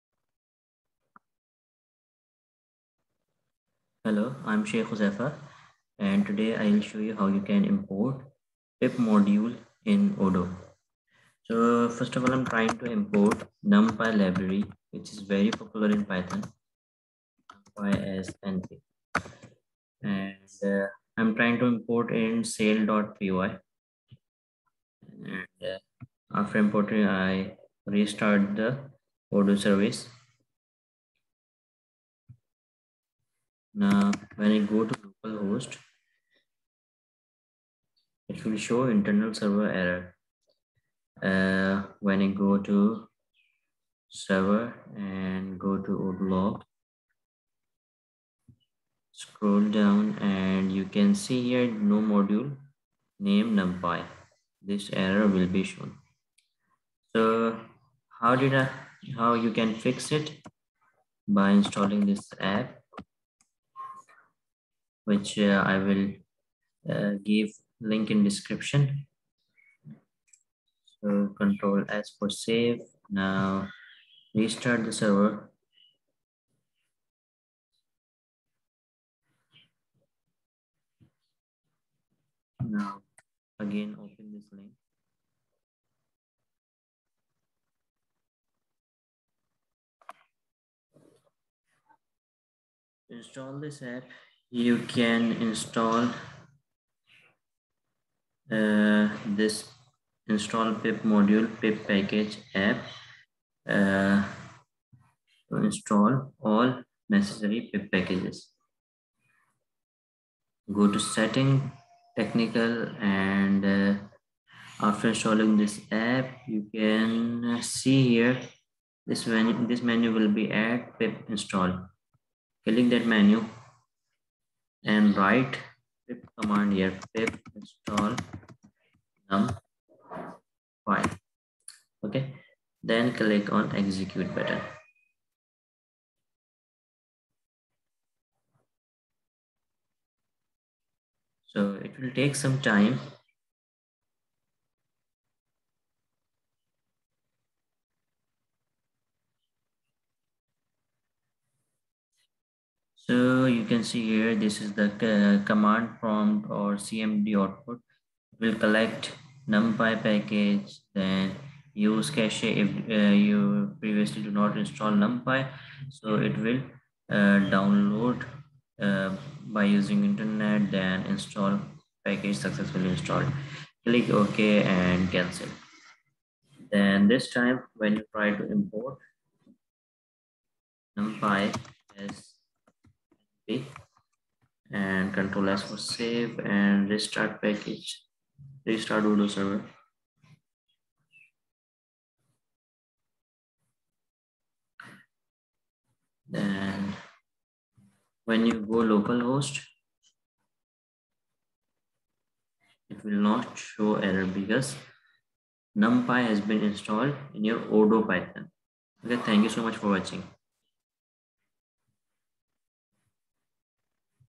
How to install PIP packages in ODOO
You can install any pip packages in odoo